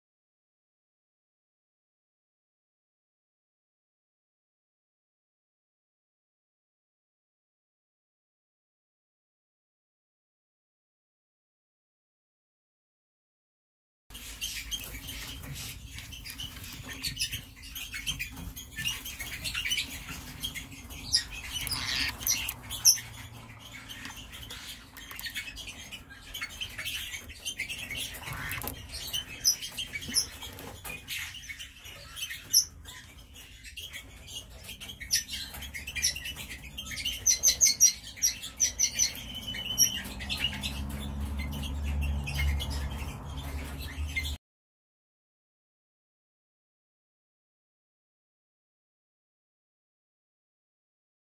Psittacoms: Citron 2019, 1er niché du B N°A3
Couple de perruche Citron avec leurs deux premier petits et avec encore  la possibilité de 5 jeunes à venir au vue des 5 œufs restants.